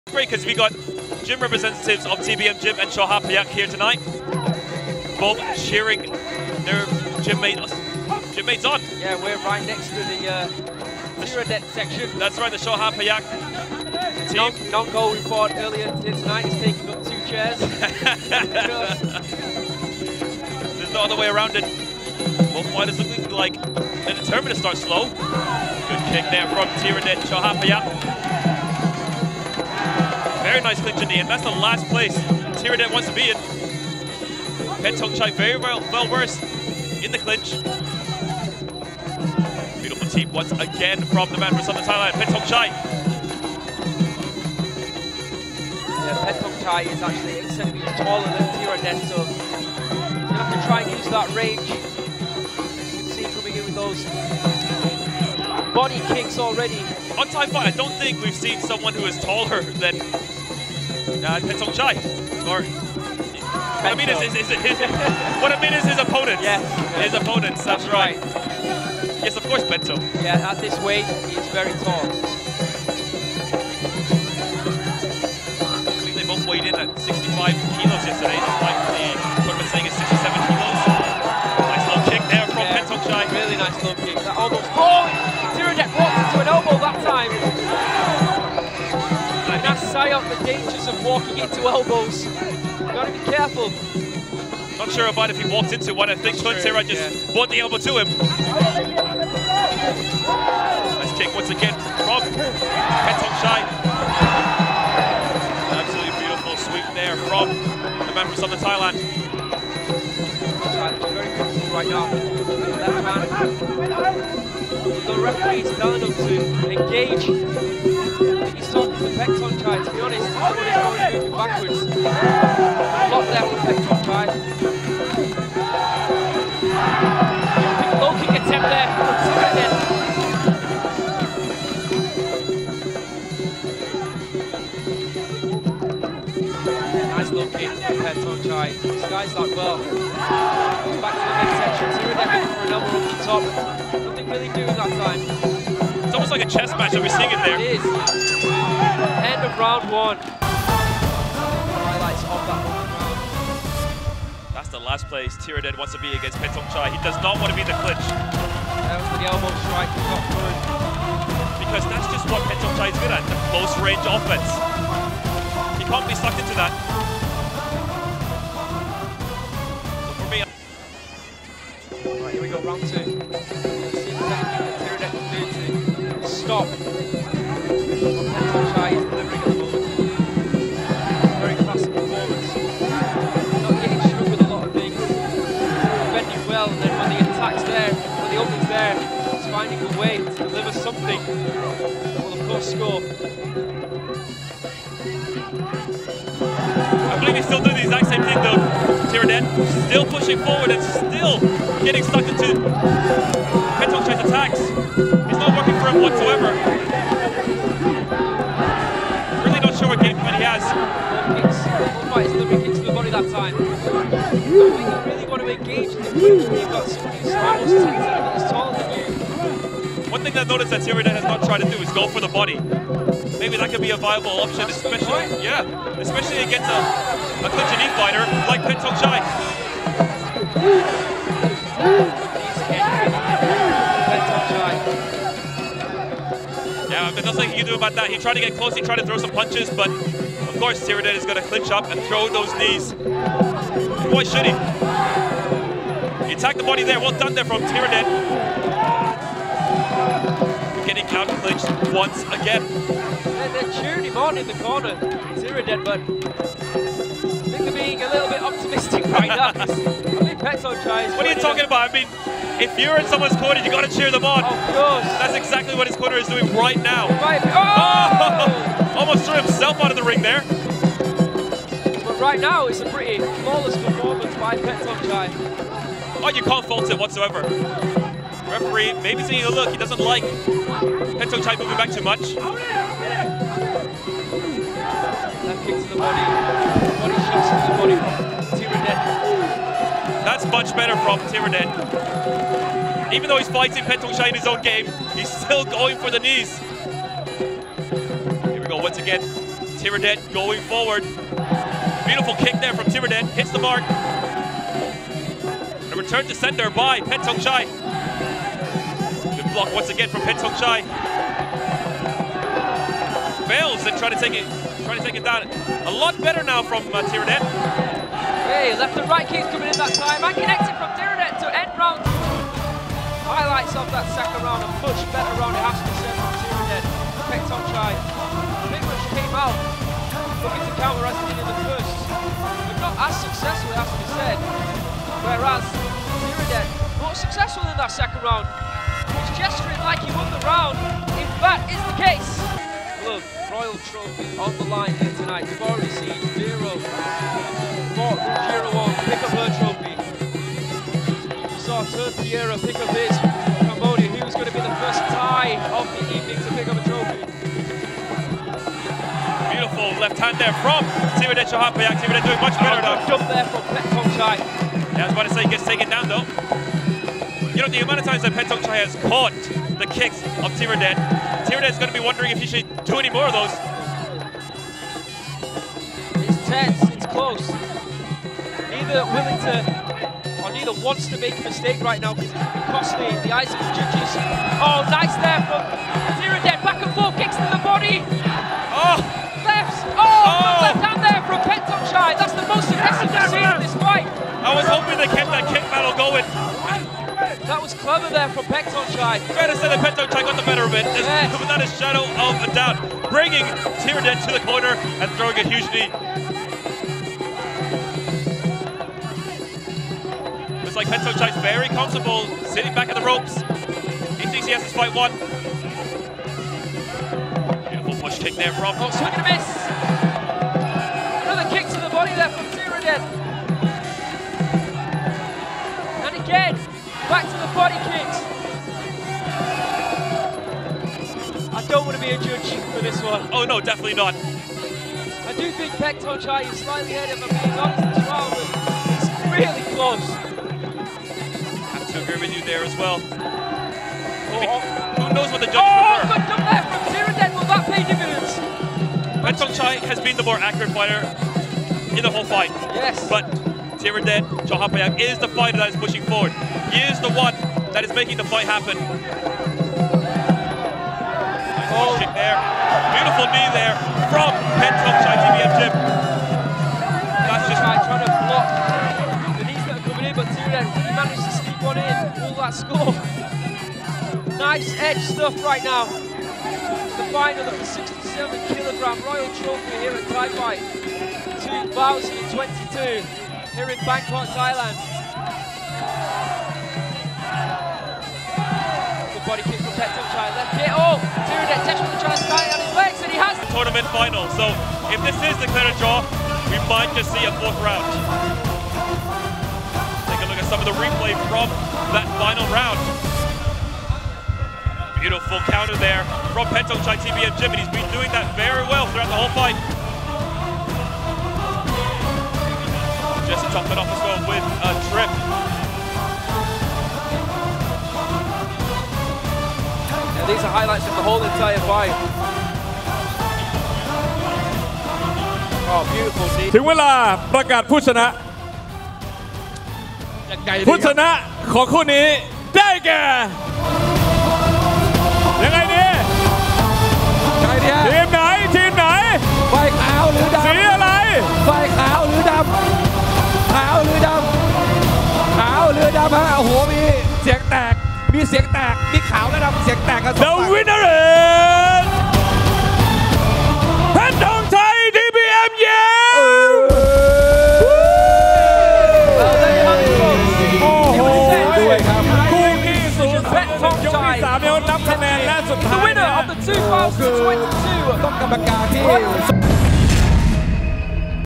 PETCHTHONGCHAI Vs THIRADECH 100 Years Nakornban Thai Fight King of Muay Thai
Thai Fight King of Muay Thai at thailand